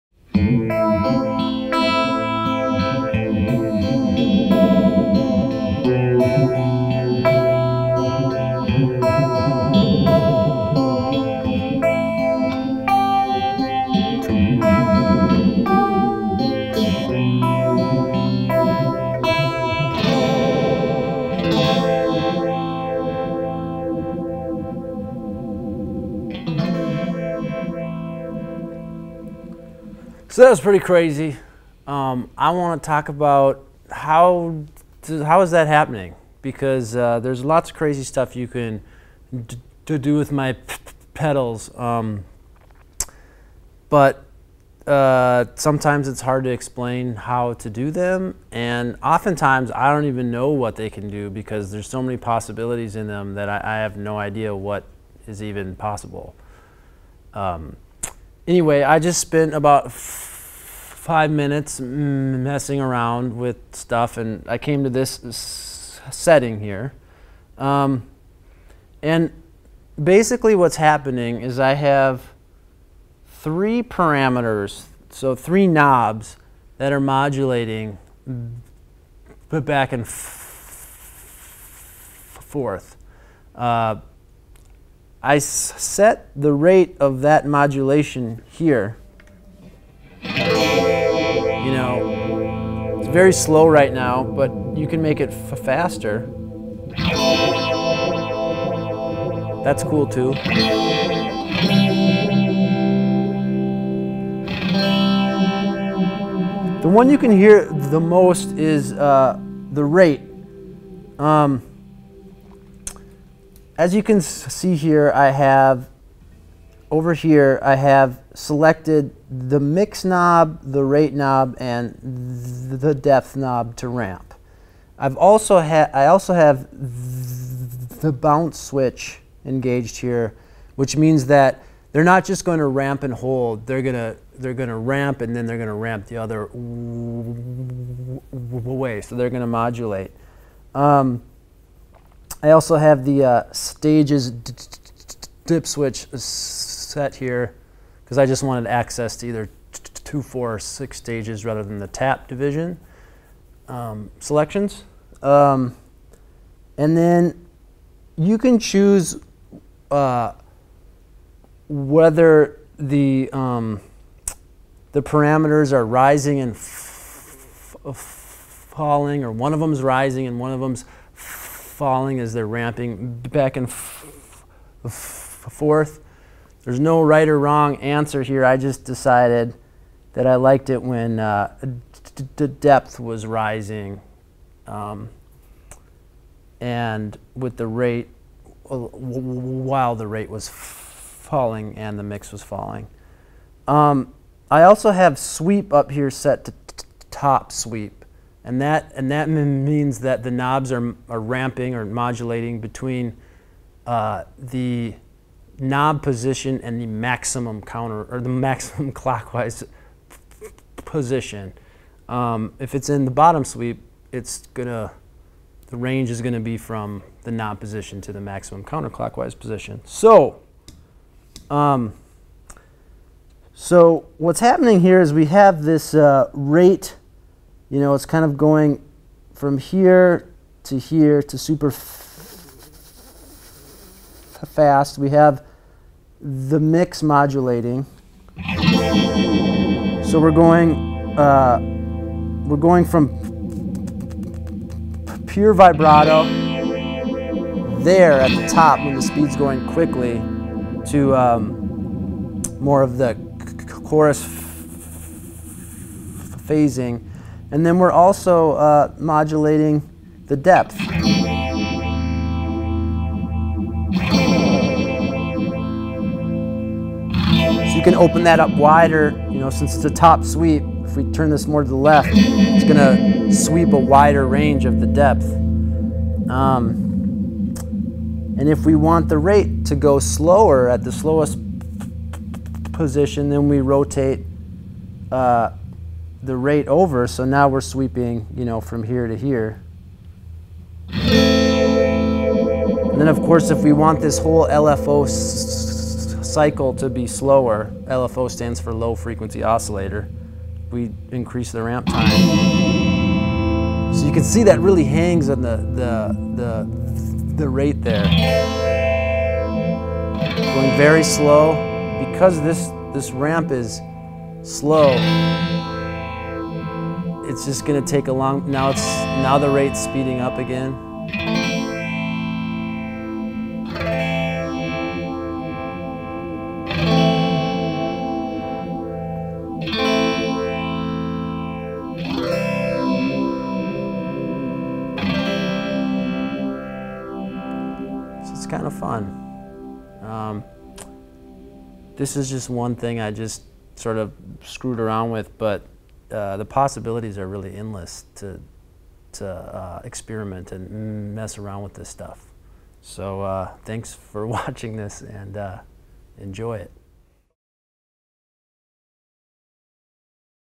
Wombtone mkII Dip Switch Lesson 1: Ramping Multiple Parameters
Hang out with Joel for awhile and explore some multiple parameter modulation with the magical ramp knob